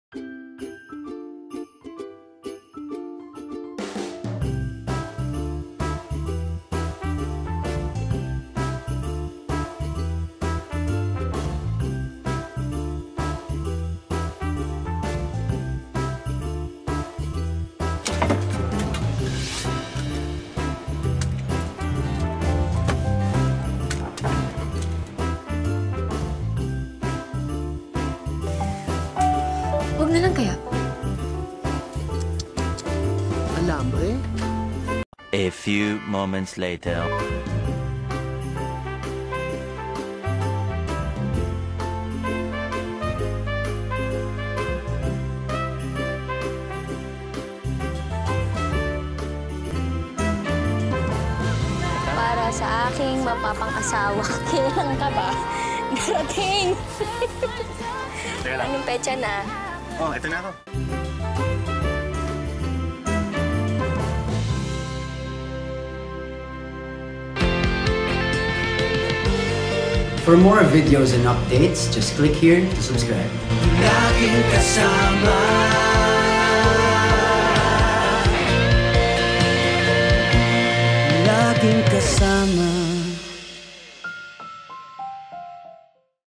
Tips Para Hindi Malamig Ang Pasko Mo | Cinema One
35 days to go before Christmas mga beshie! May munti kaming paalala para sa inyo: anuna te?

Galaw galaw din kung ayaw mapa-bilang sa SMP. 🥶